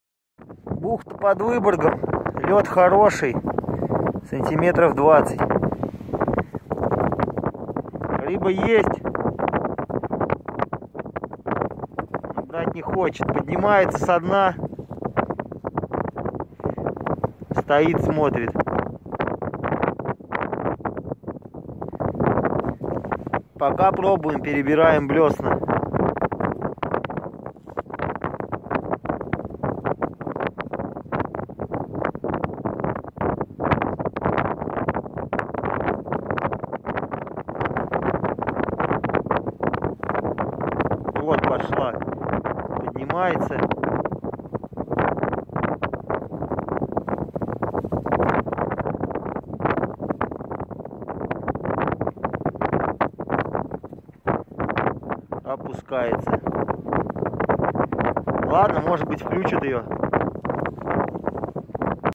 эхолот практик видит рыбу...
рыба поднимается но не берет